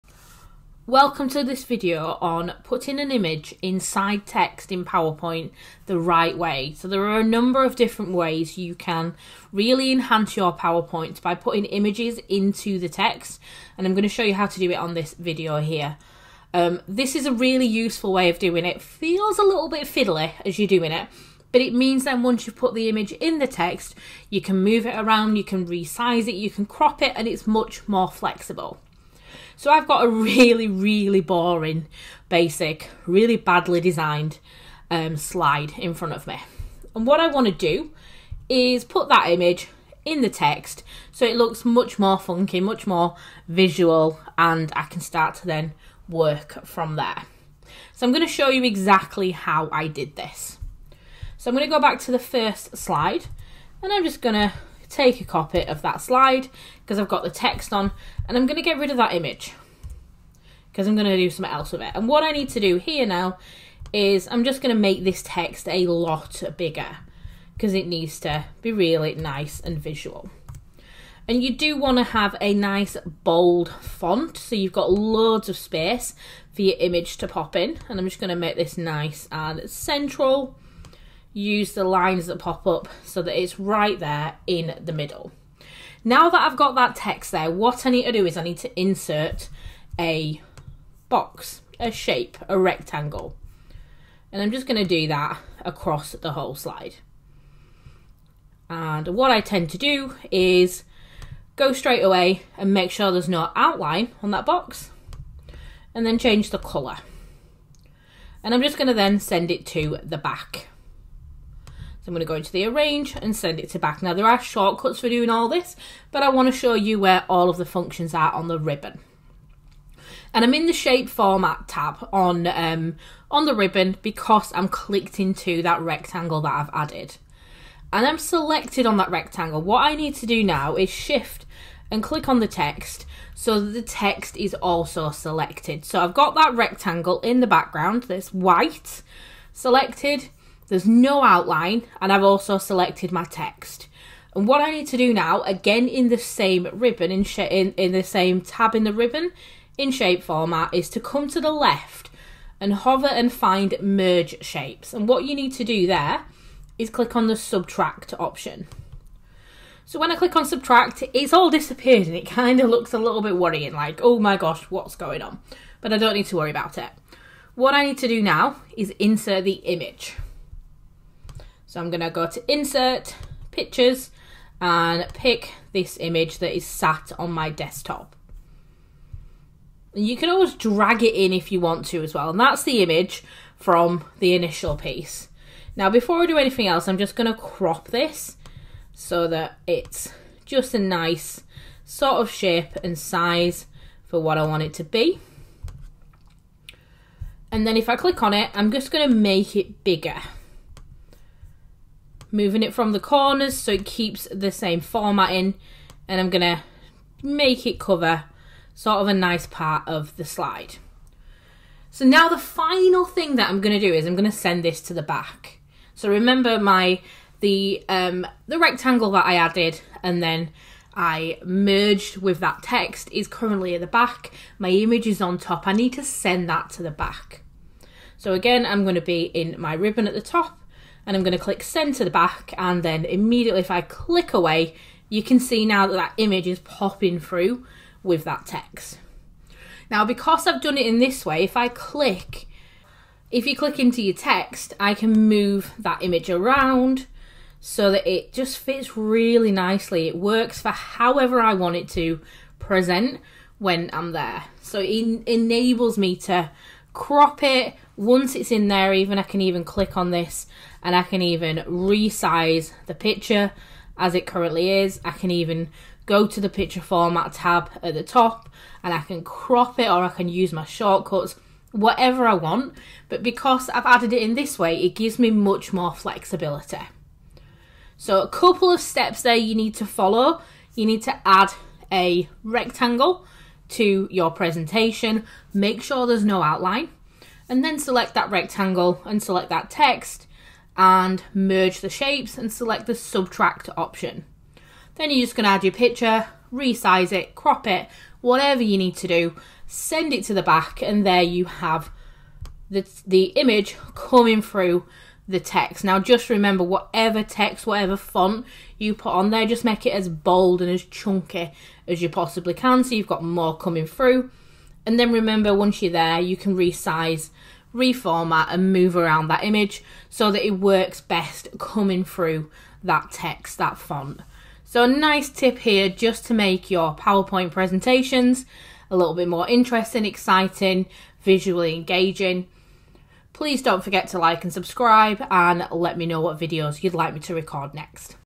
Microsoft PowerPoint - Adding an image into text
In this video, we're going to have a look at how to add an image into text the right way in Microsoft PowerPoint. Following this process will allow you to manipulate the image once it's been added.